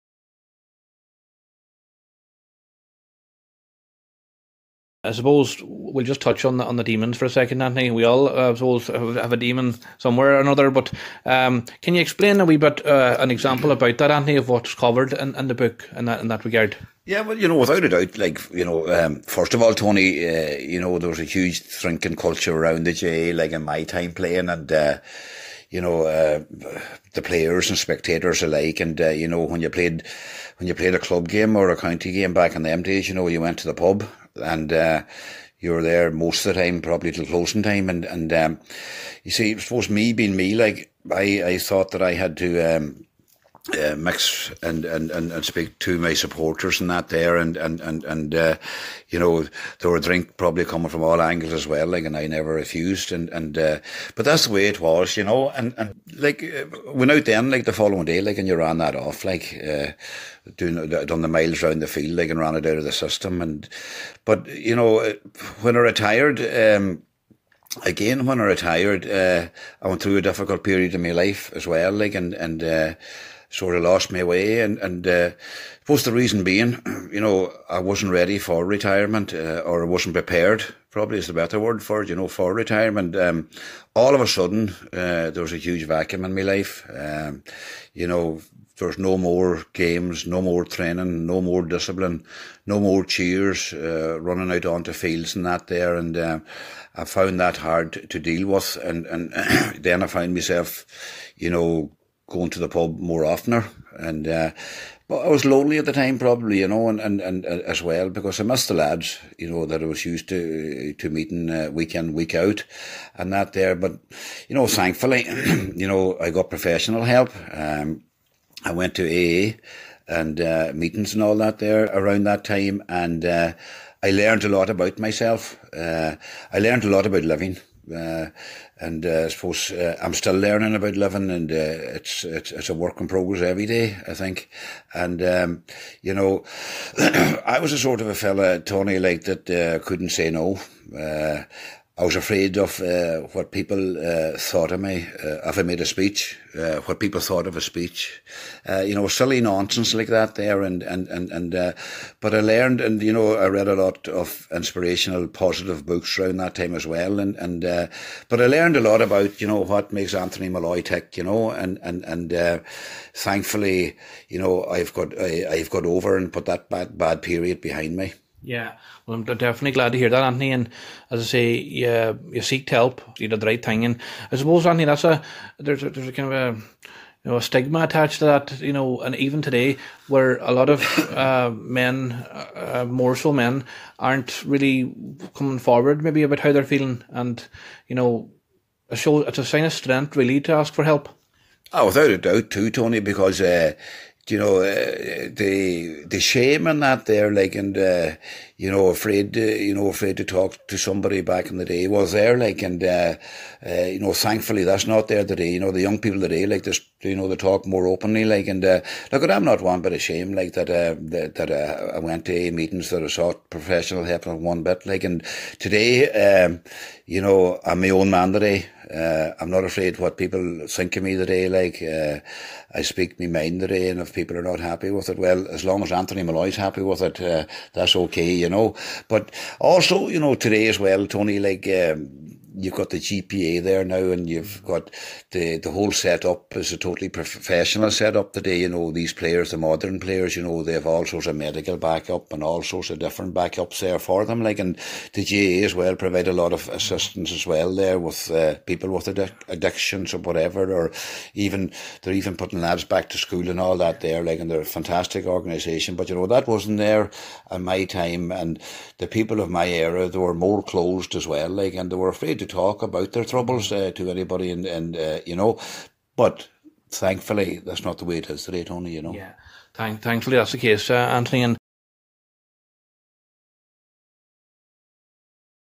Anthony Molloy On Demons Faced During & Post GAA Career
When I featured former All Ireland winning captain with Donegal Anthony Molloy on my podcast (EP 72) I talked him about some of things covered in his memoir called 'Life, Glory & Demons'. This clip covers the part about the demons; his battle down through the years.